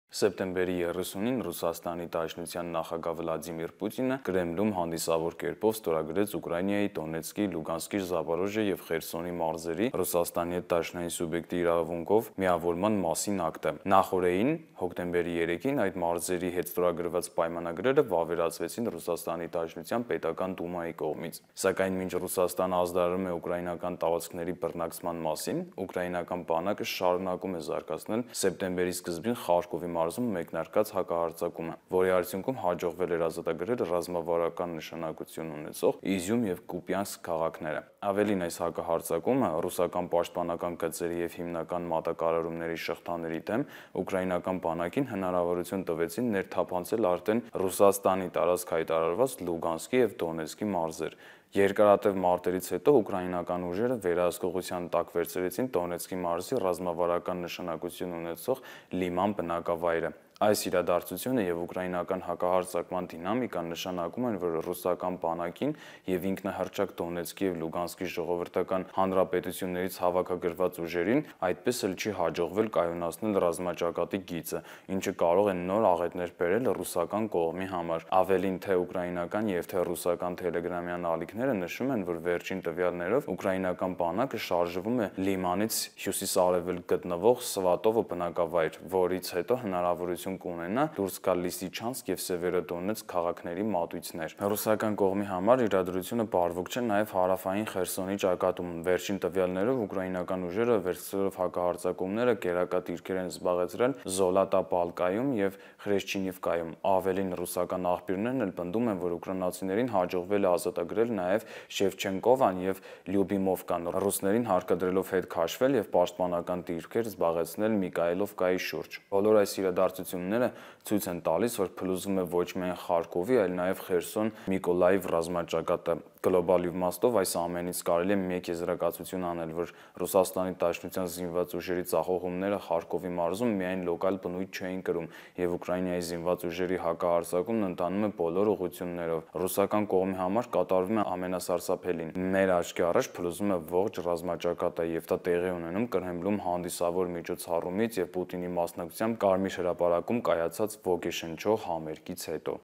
Ուկրաինայի զինված ուժերի հակագրոհների արդյունքում ռուսական ռազմաճակատն իր ողջ երկայնքով փլուզվում է
- Եթե ցանկանում եք ստանալ թարմ նորություններ, օբյեկտիվ ու օգտակար տեղեկատվություն, բաժանորդագրվեք մեր Newarmenia.am ալիքին `